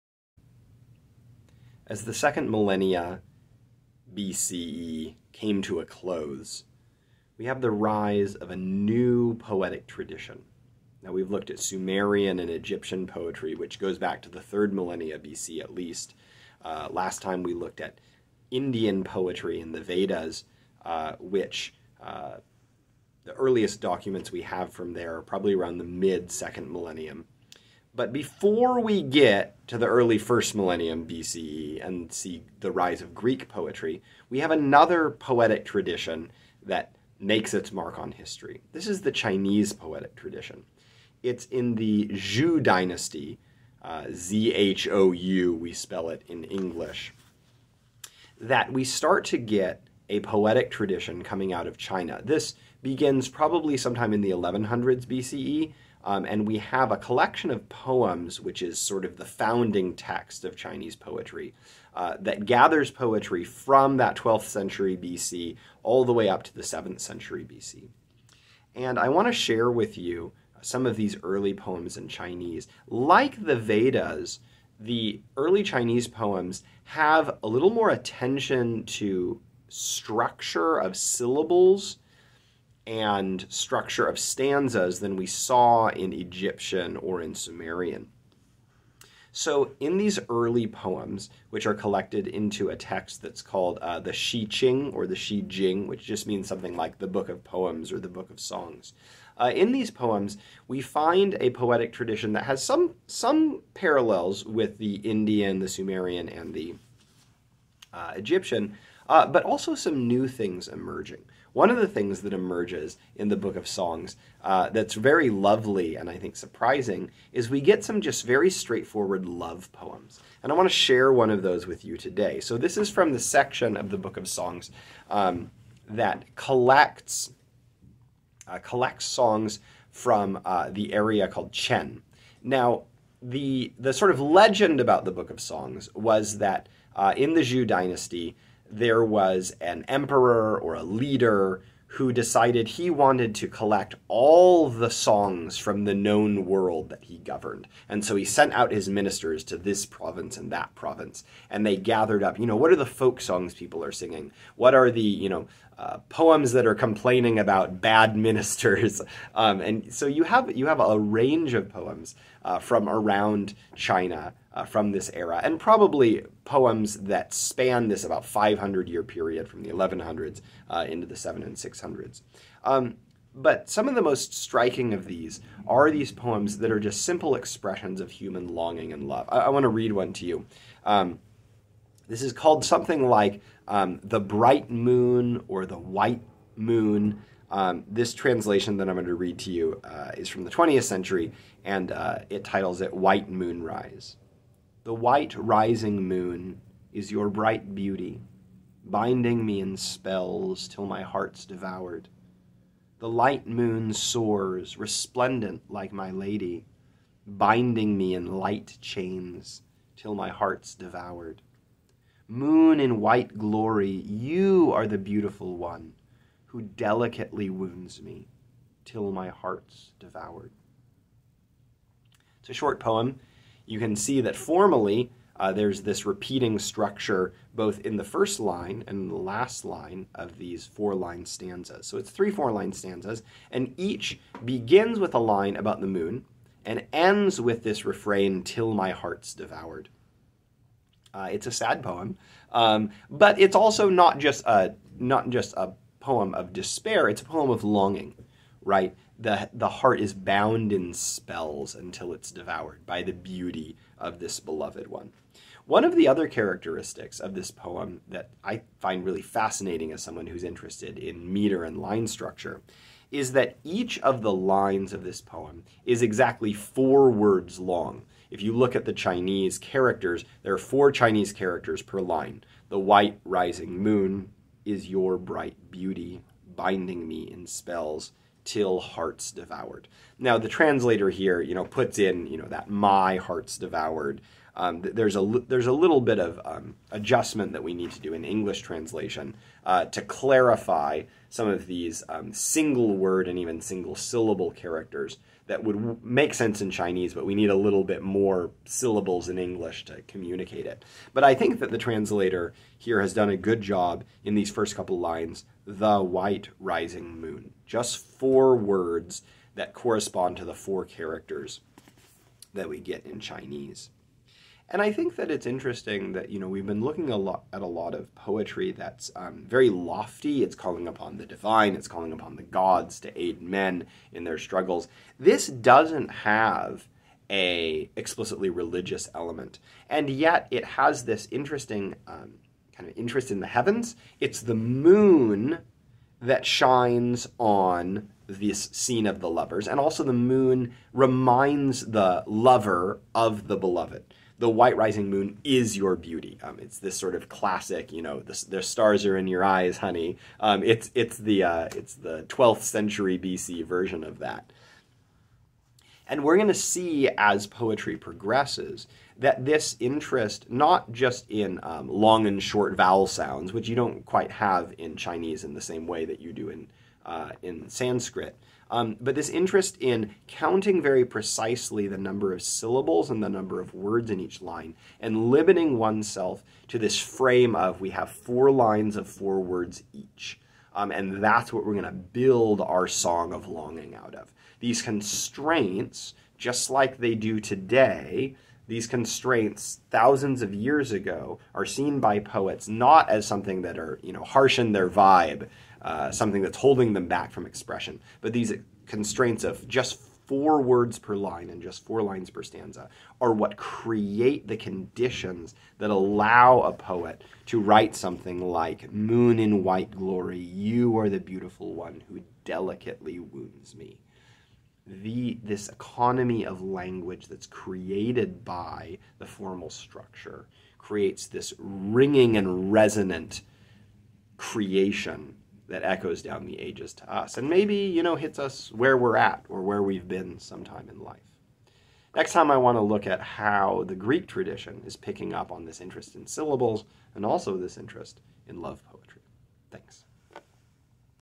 Poetry in Ancient China
The poems of Zhou Dynasty China from the Shih Ching (Book of Songs) give us a glimpse of love, longing, and formal rigor.